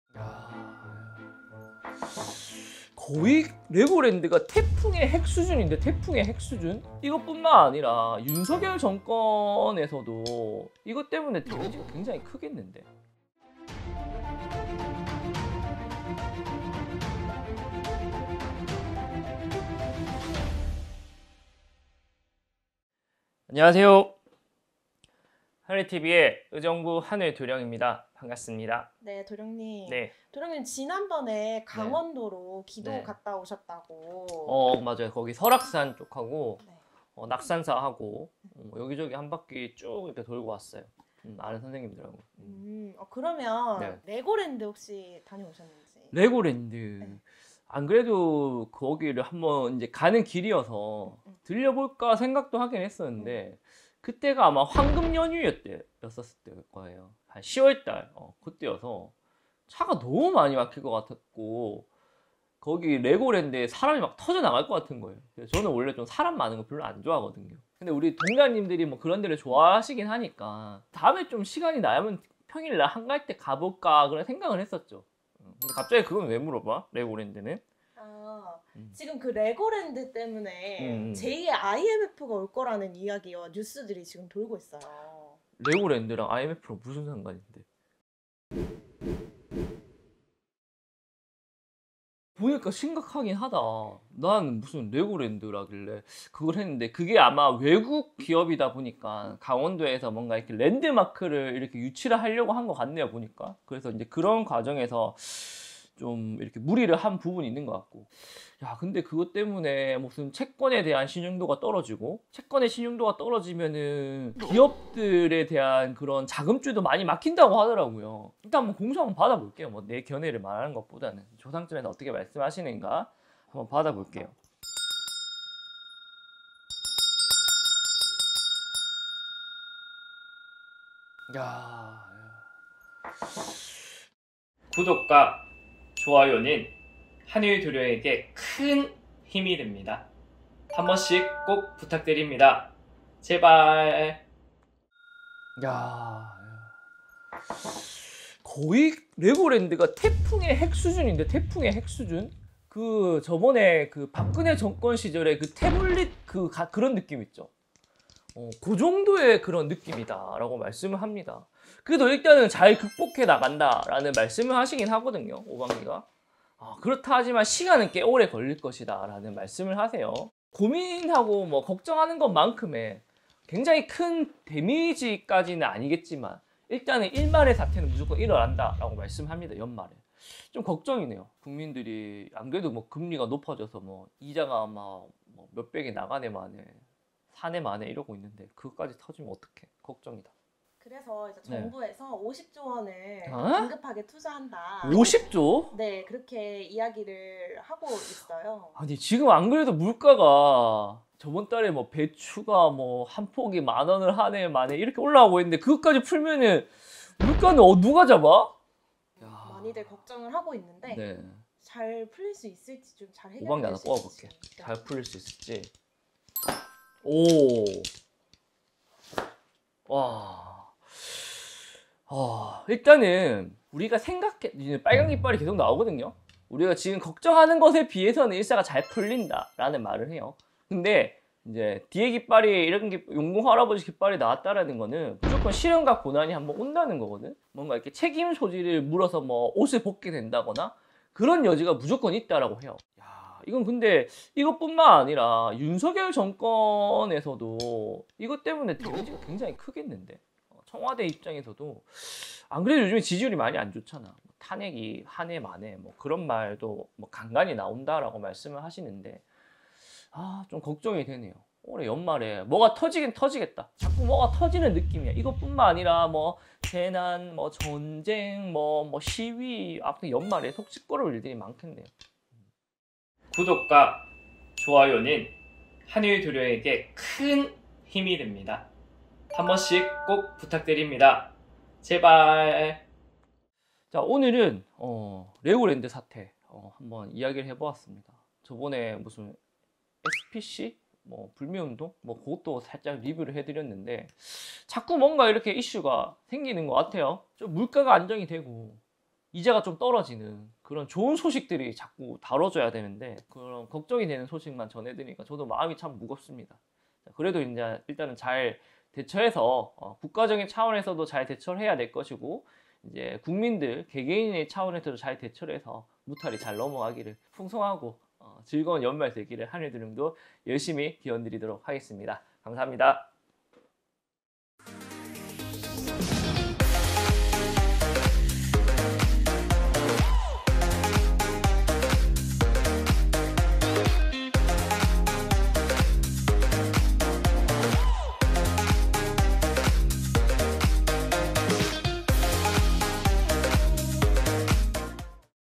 역대급 대형 사고! 레고 랜드 사태, IMF보다 더 심각한 경제 위기? / '진태' 양난 속 등 터지는 윤 대통령 이대로 탄핵까지? / 국운 예언
역대급 대형 사고! 레고 랜드 사태!
IMF보다 더 심각한 경제 위기?
'진태' 양난 속 등 터지는 윤 대통령!
이대로 탄핵까지?